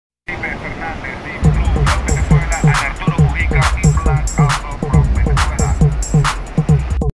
Resumen Día 3 @bodyboardpf 2012
campeonato mundial 2012 @bodyboardpf / playa parguito NE del 20 al 23 sep 2012